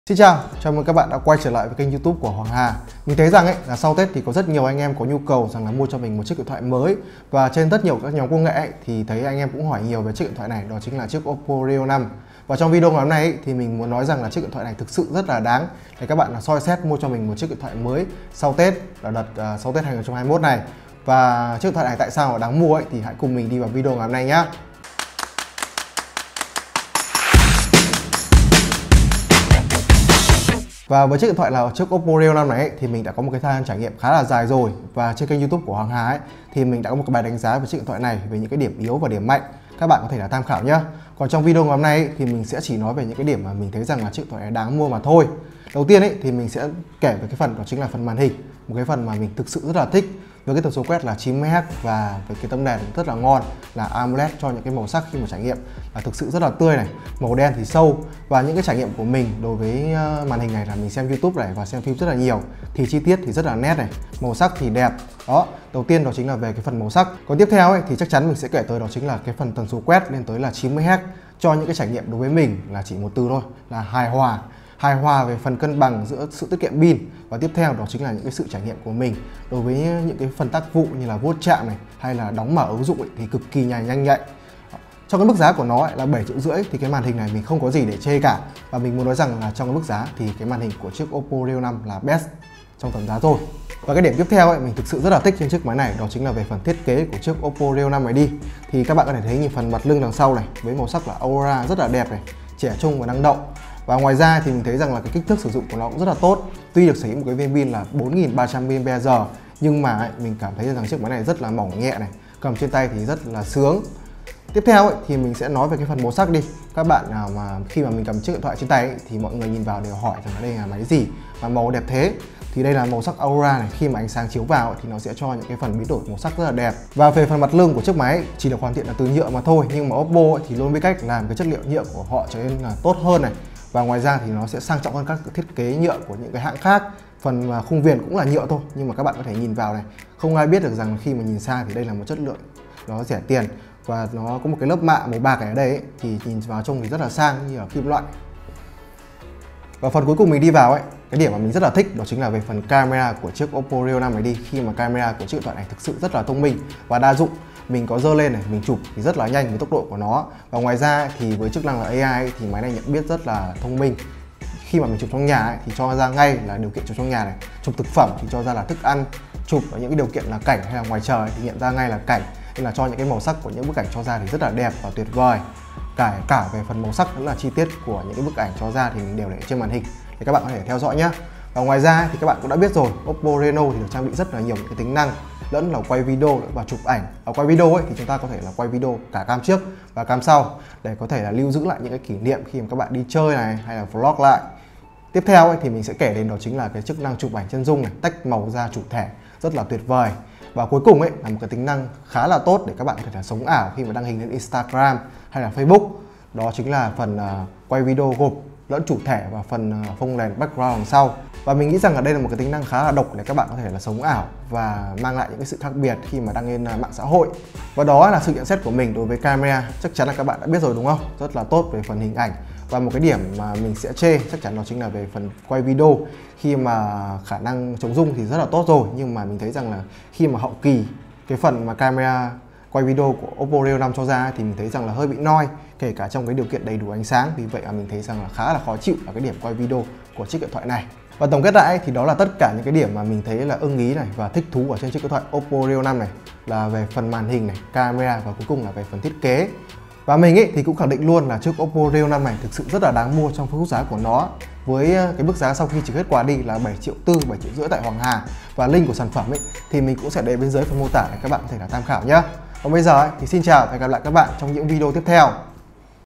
OPPO RENO5 - Chiếc điện thoại đáng mua nhất đầu năm 2021
Chính thức ra mắt tại Việt Nam vào ngày 30/12, Oppo Reno5 (4G) dự kiến chỉ là cái tên đầu tiên của dòng Reno5 cập bến các thị trường bên ngoài Trung Quốc, trong khi các phiên bản 5G được dành cho đầu năm 2021. Mức giá 8,69 triệu đồng cho phiên bản 8GB RAM và 128GB bộ nhớ trong của Oppo Reno5 là không chênh lệch quá nhiều so với các thế hệ trước, bằng với Reno3 và chỉ hơn Reno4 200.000 đồng ở thời điểm ra mắt